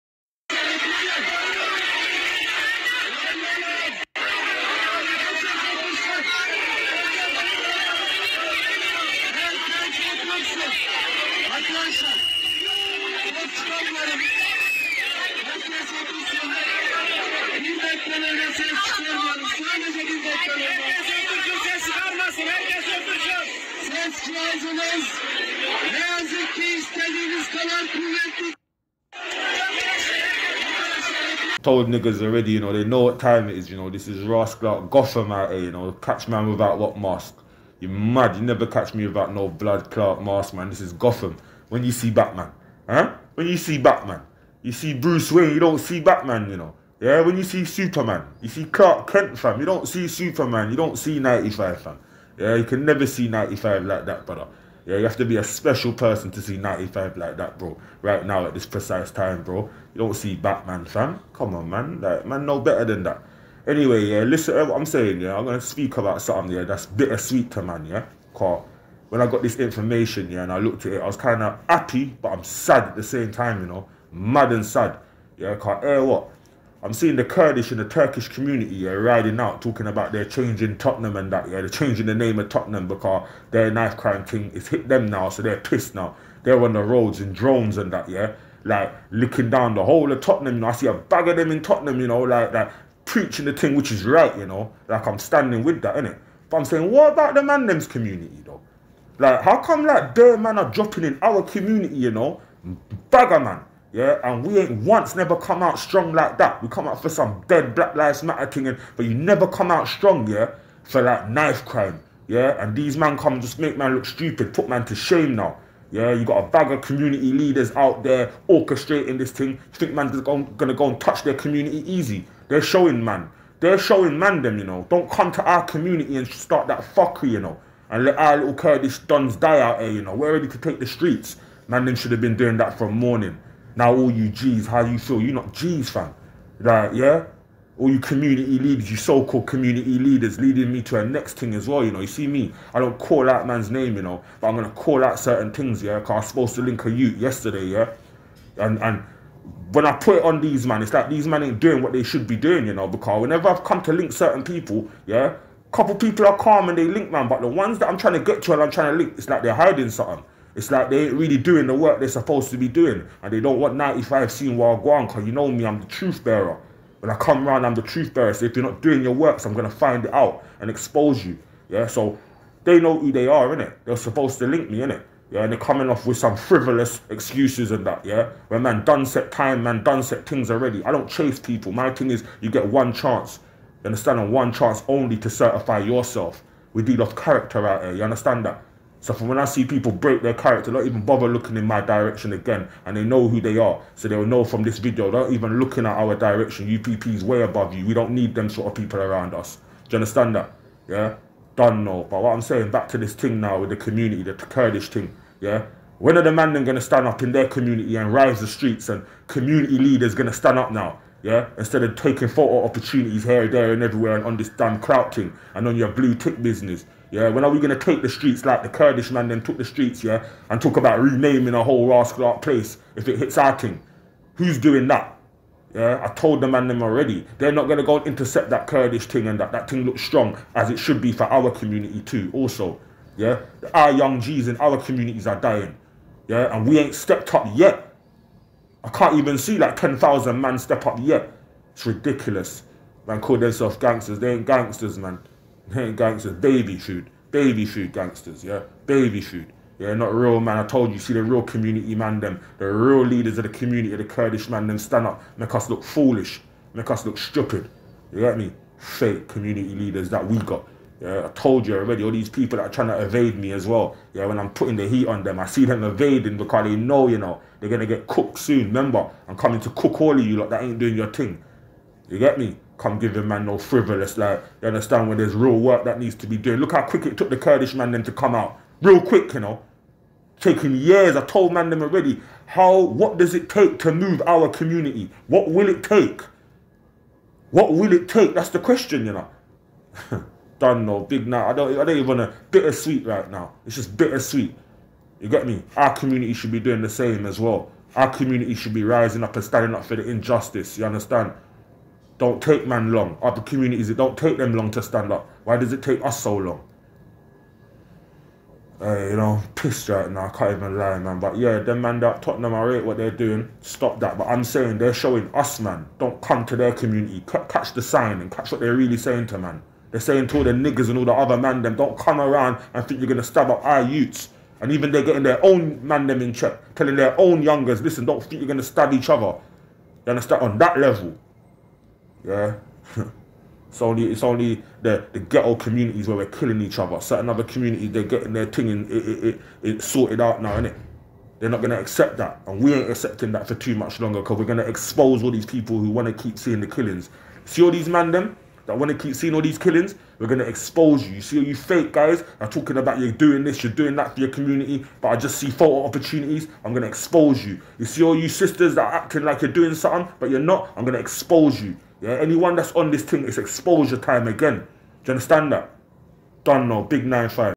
KURDISH MANDEM STORM TOTTENHAM HIGH ST (TURKISH) RECLAIMING THE STREETS / WHAT ABOUT US
KURDISH MANDEM STORM TOTTENHAM FOR KNIFE CRIME YOUTH DEAD... RECLAIMING THE COMMUNITY.. WHAT ABOUT OUR COMMUNITIES... @95CRIPGBMBOFFICALMEDIA peckham upp savetheyouth CALLING OUT THE NO SHOWERS